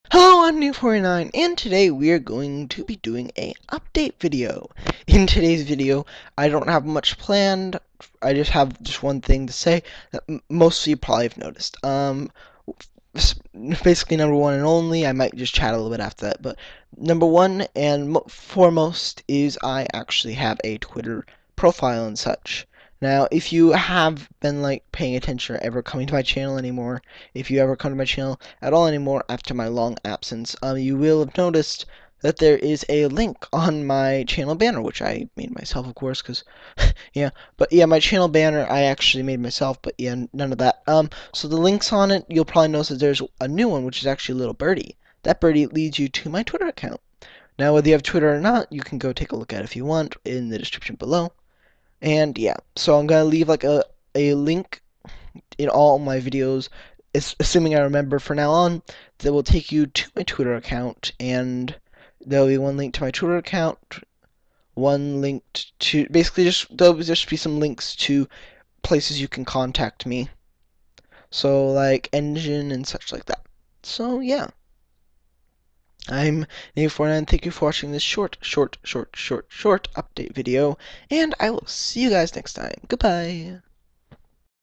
Updates! | I Have Twitter!
Hay Guys! Nibby here, and today I bring you a very short update! In todays update I just Mention New My Twitter Account!